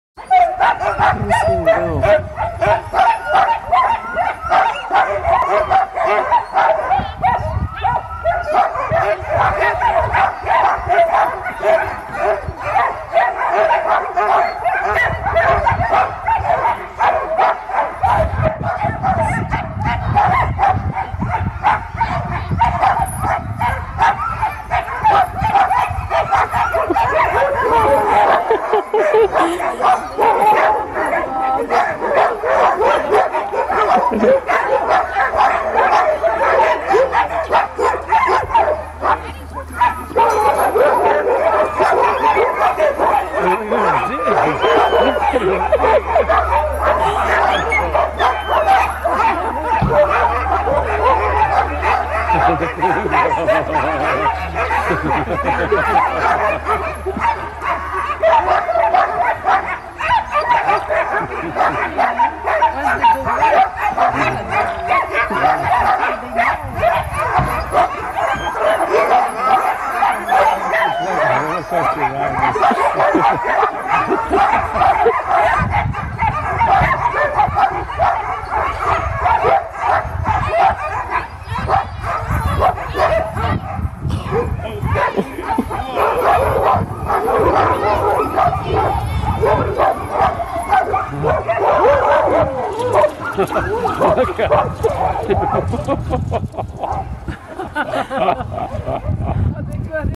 video skagway bus caribou crossing sled dogs takeoff DSCN0962
Dog sleds taking off at Caribou Crossing in Yukon Territory.